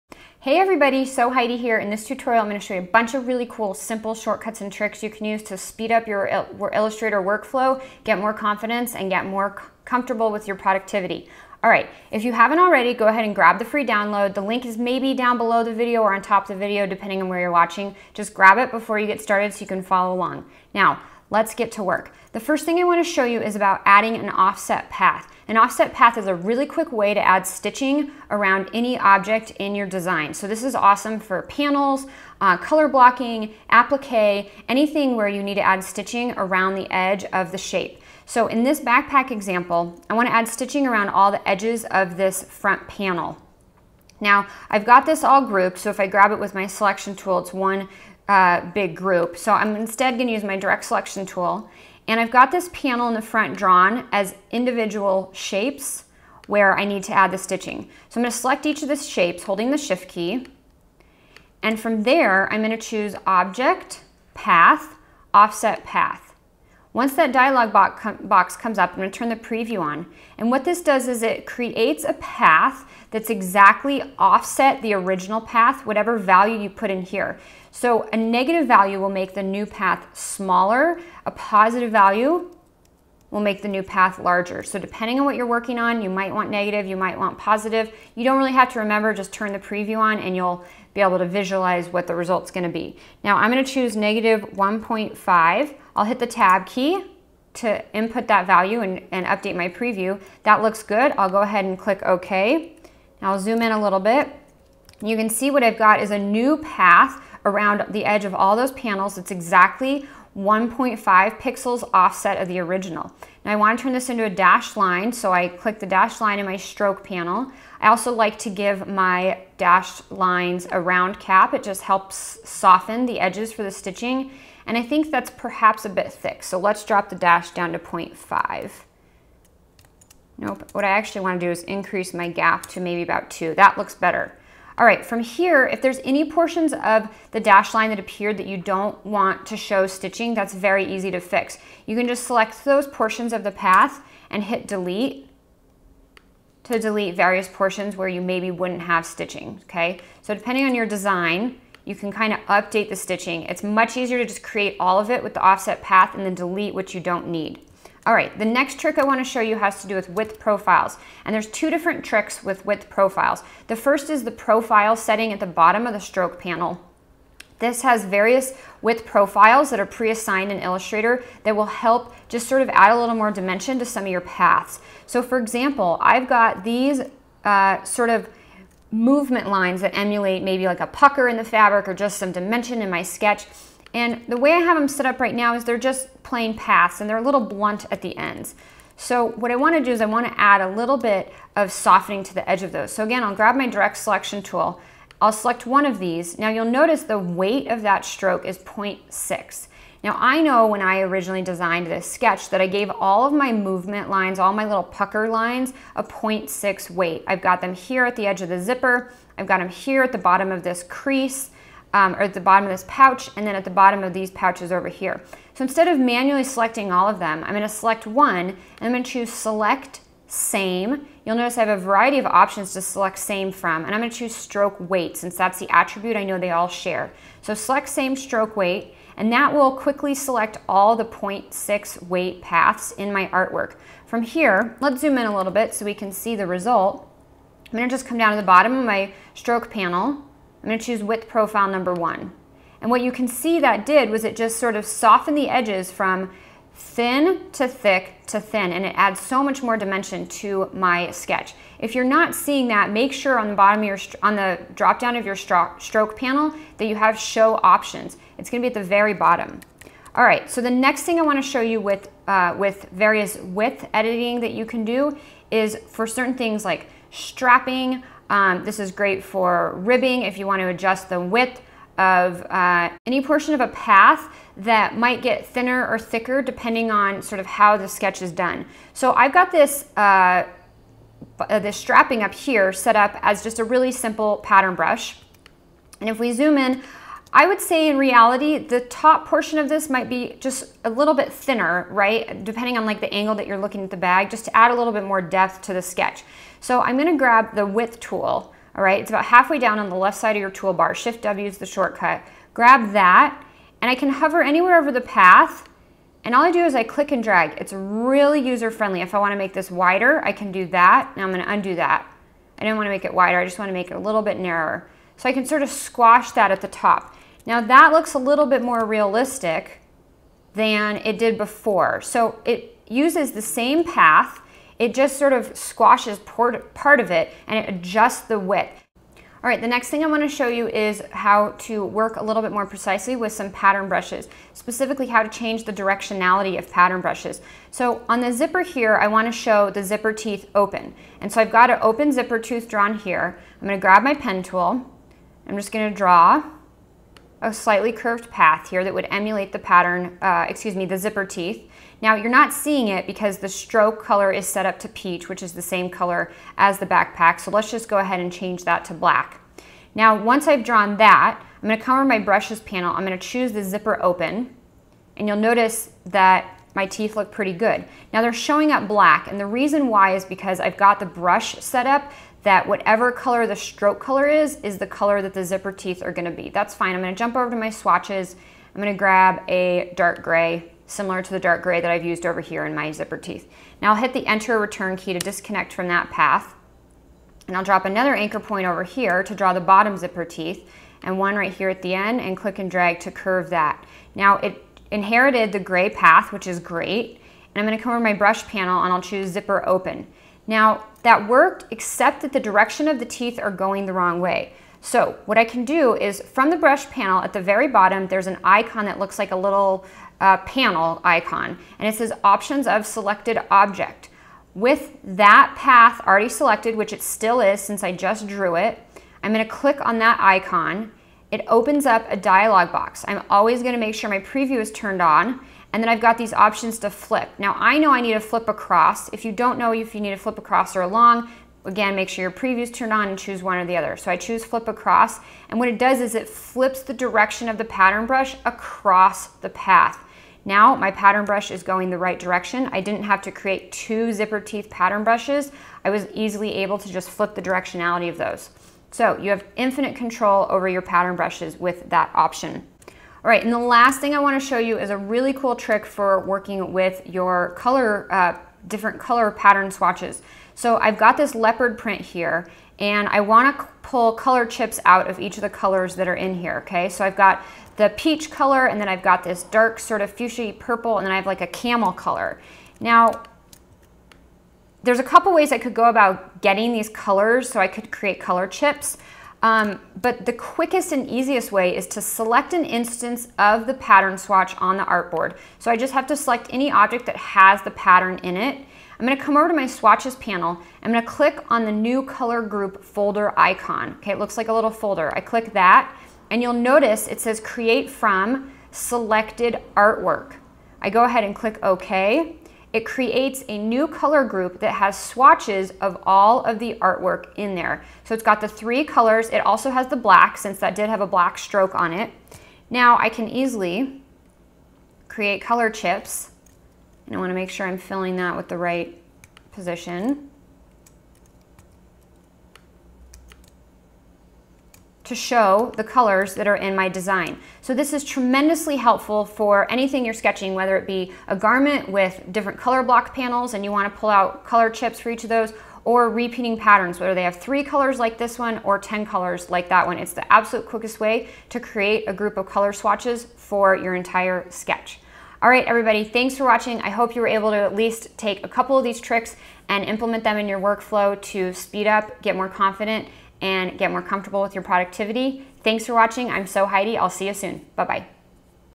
Sketch Fashion Flats Faster with these 5 Illustrator Shortcuts
In this video, you will learn how to:

00:52 Add stitching to your fashion flat with this simple hack
02:37 Create stylized movement lines by changing one setting
04:19 Change the width of a path from thick to thin and back again
05:28 How to change the direction of pattern brushes (zippers, stitching, etc) without creating a new brush
07:32 How to create color chips / color callouts quickly from pattern swatches